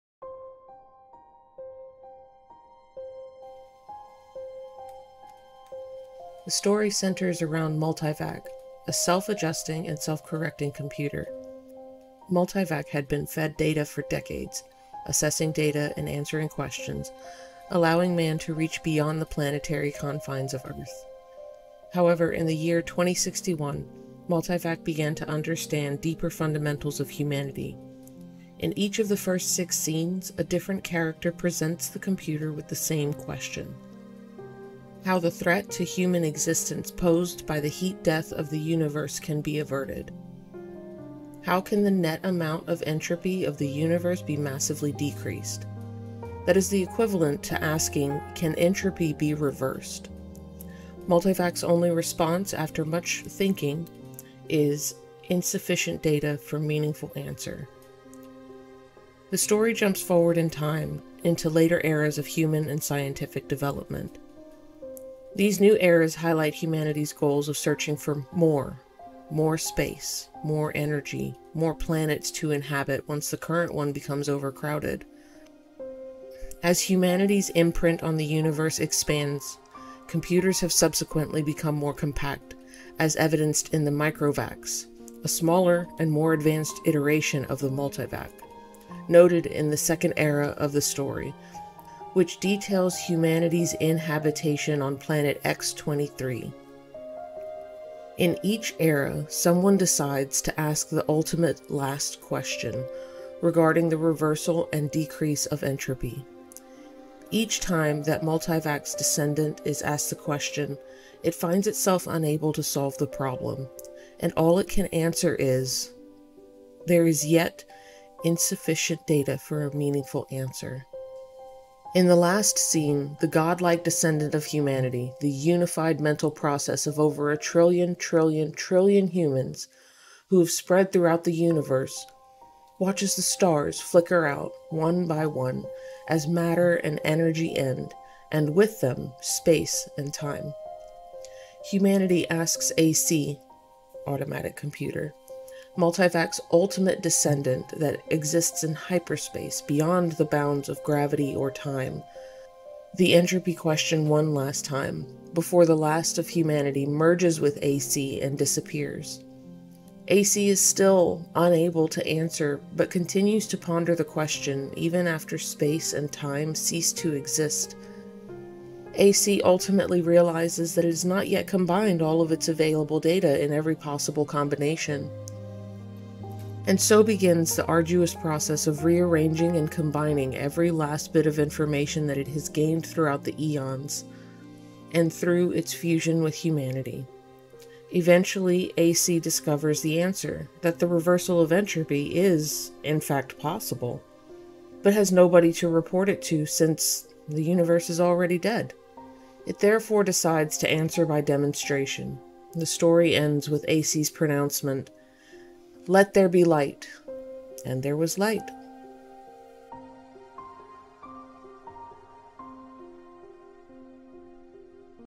Isaac Asimov's "The Last Question" Summary
In the Last Question generations of technicians and scientists repeated as a super computer "Why did entropy reverse itself?", to which the super computer didn't have an answer for initially.

Famous short story by Sci Fi Author Isaac Asimov summary narration. Asking the age old question of why did entropy reverse itself at the beginning of time? It wouldn't be until the end of time when the multivac super computer arrives at an answer.